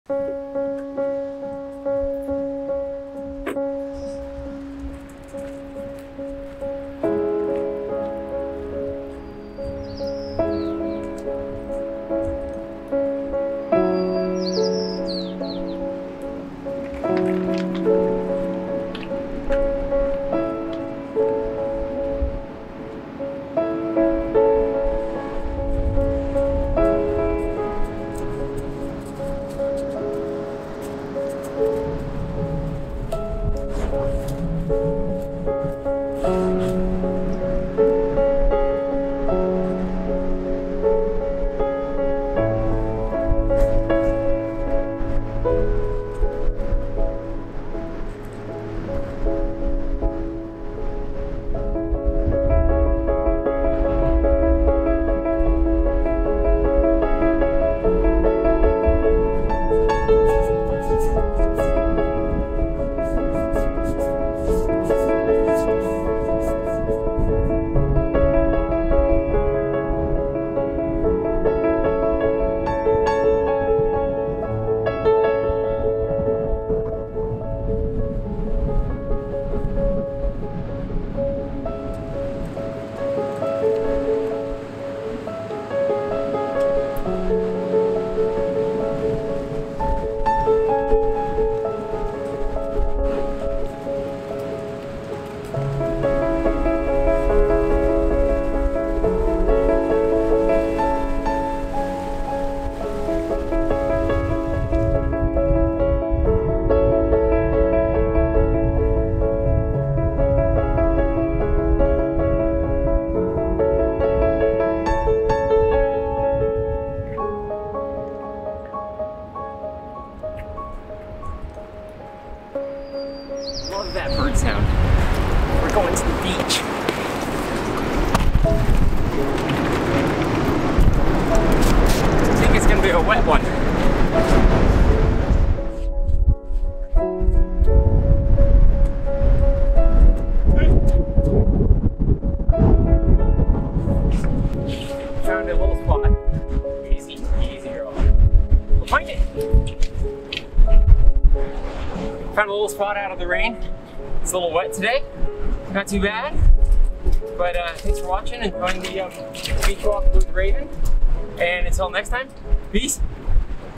A Rainy Beach Walk on the Oregon Coast | Arcadia Beach State Park
Join us on a peaceful beach walk on the Oregon Coast with Raven the black lab dog at Arcadia Beach State Park, just south of Cannon Beach.

Despite the wet and rainy weather, Raven's joyous enthusiasm for exploring the beach and chasing her tennis ball is sure to brighten your day.

Watch as we take in the sights and sounds of the Pacific Ocean, admire the wildlife, and enjoy the solitude of this beautiful state park.

Recording Date: April 8, 2023